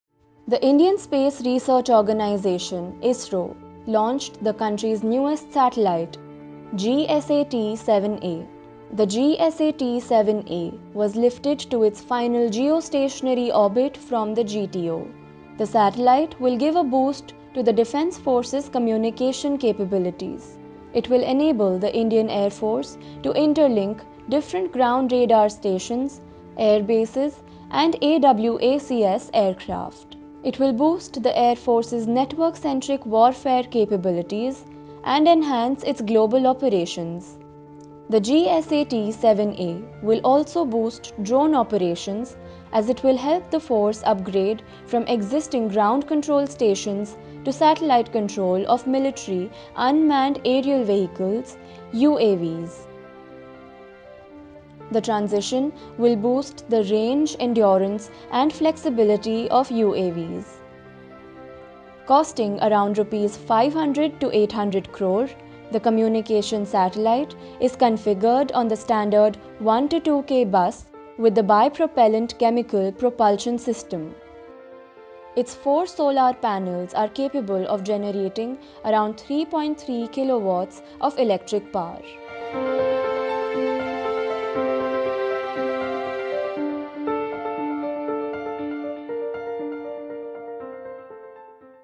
ISRO Launched Military Communications Satellite GSAT-7A
The Indian Space Research Organisation (Isro) on Wednesday, launched the country’s newest satellite GSAT-7A to boost the defence forces’ communication capabilities.